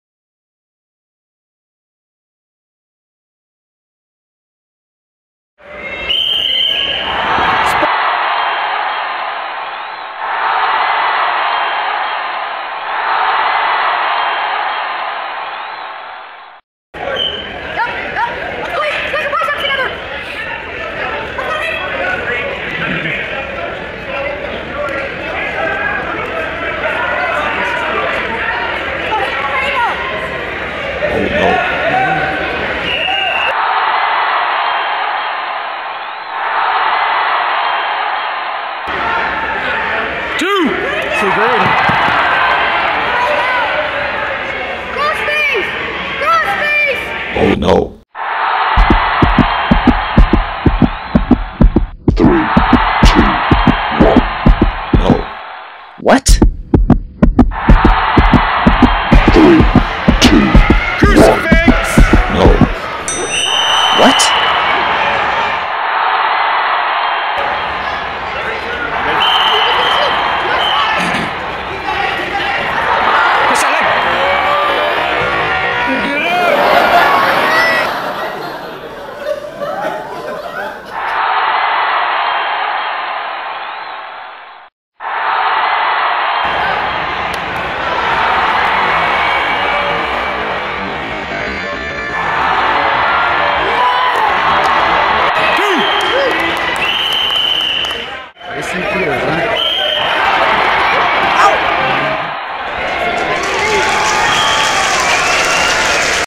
Standing Cradle To Crucifix Pin Youth Wrestling Tournament
Muscle vs Technique

Hit a Standing Cradle And Finish With A Crucifix

He wants more?
Keep your eye on the prize...

"He shoots, I score. I shoot, I score."
Double Leg Take Down!

youthwrestling 12 and under 94lbs